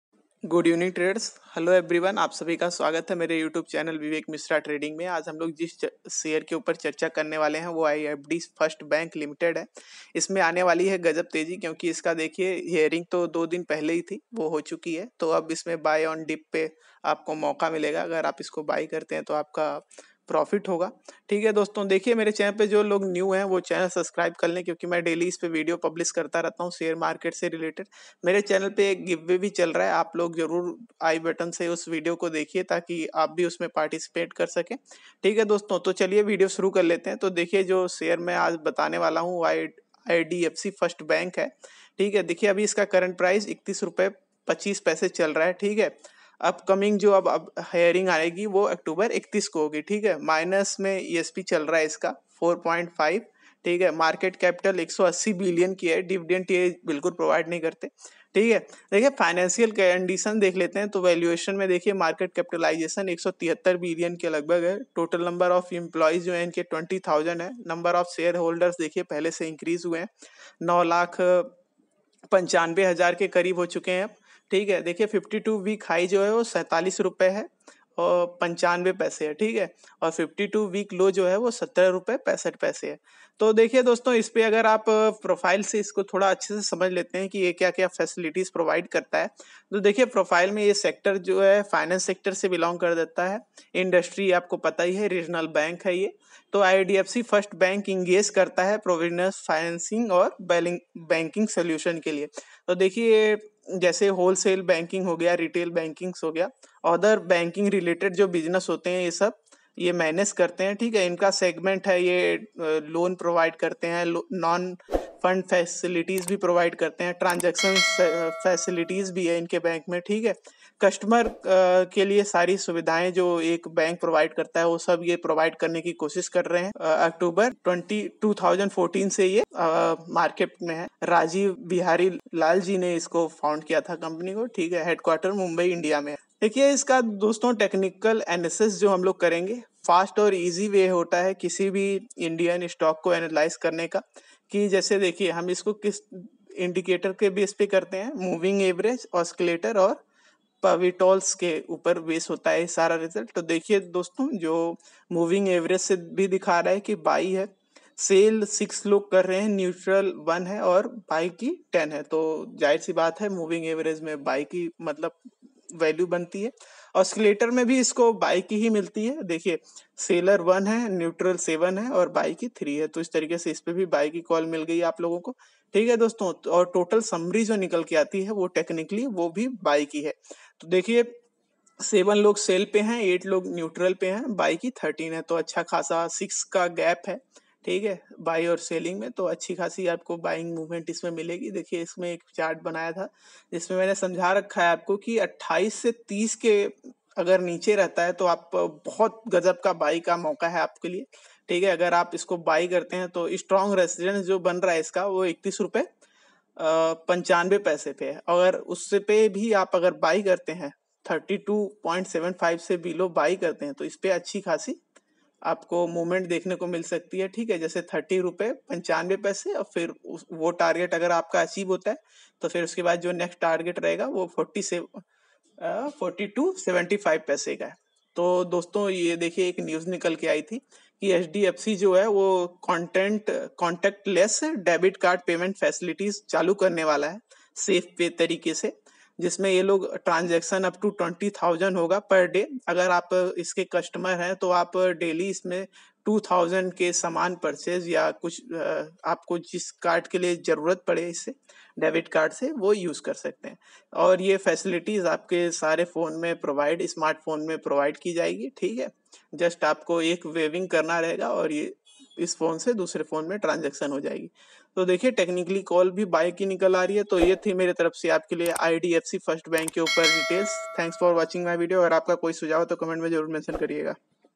IDFC First Bank Ltd Share Buy on Dip at Current Price Indian stock market NSE: IDFCFIRSTB
IDFC First Bank is an Indian banking company with headquarters in Mumbai that forms part of IDFC, an integrated infrastructure finance company. The bank started operations on 1 October 2015. IDFC FIRST received a universal banking license from the Reserve Bank of India in July 2015.

Grateful!😇

idfc first bank share tamil,
idfc first bank share telugu,
hdfc bank share price target,